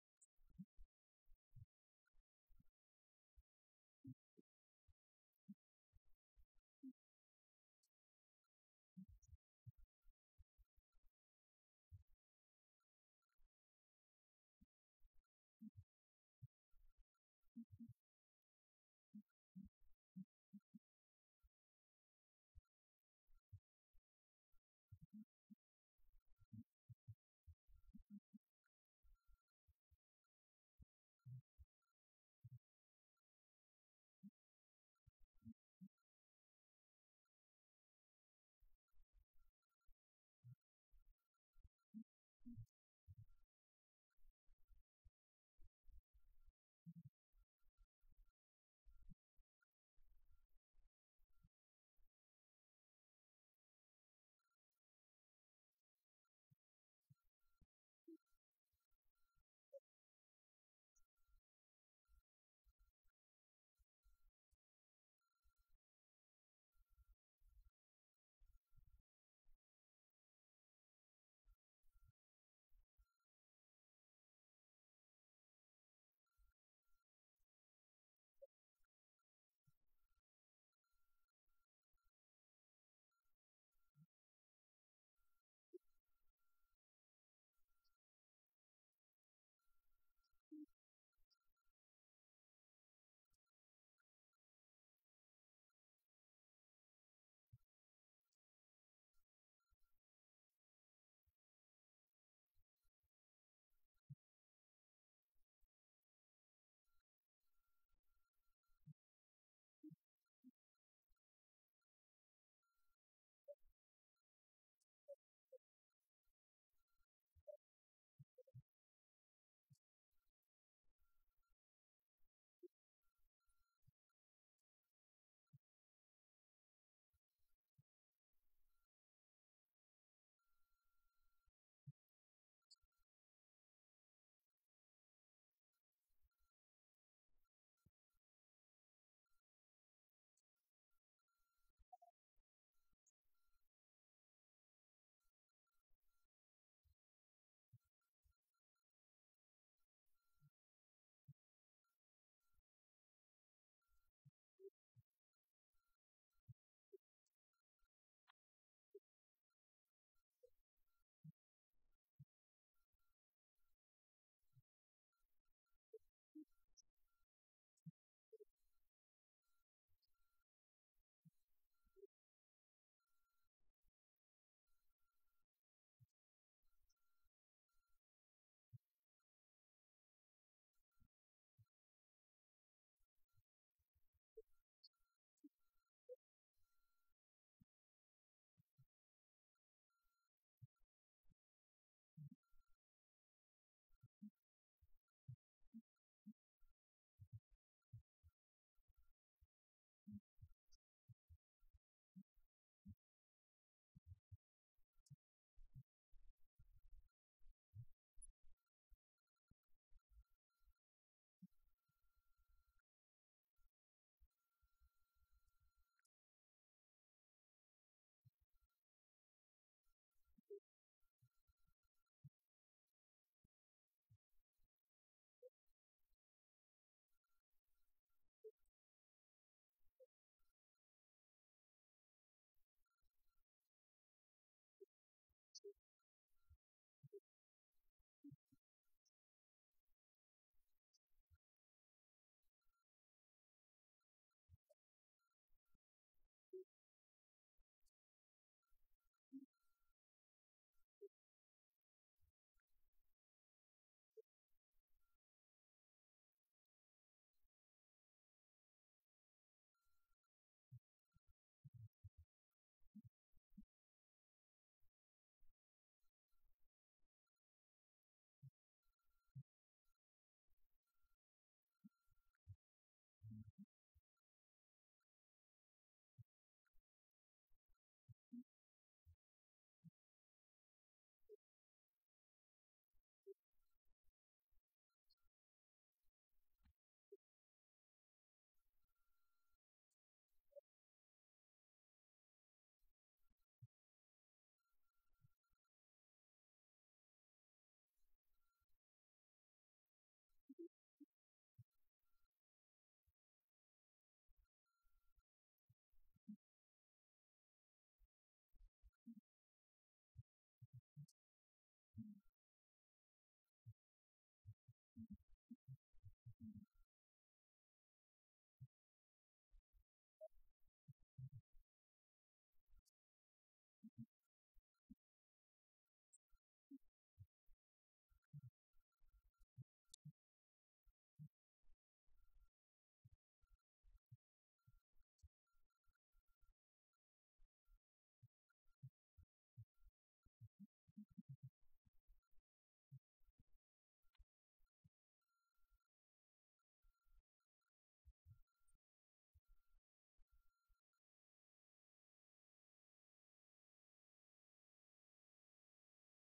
Islamic Perspective on the Holy Trinity - Part (2/3) (English)
Islam Ahmadiyya